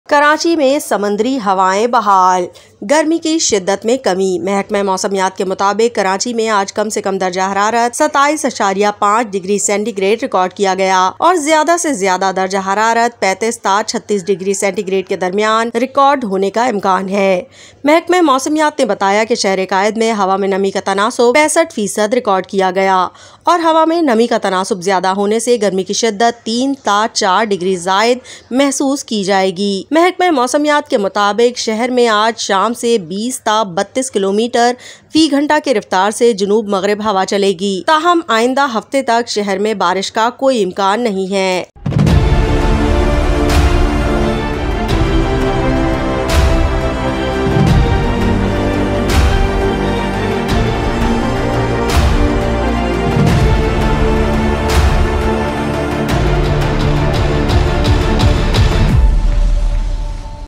Sea Breezes restored? | Karachi weather latest update | Meteorological department prediction
Sea Breezes restored? | Karachi weather latest update | Meteorological department prediction| SuchExpressNews Official

Karachi residents can breathe a sigh of relief as sea breezes return, alleviating the intense heat. Join us as we explore the impact of this natural phenomenon on the city's climate and residents.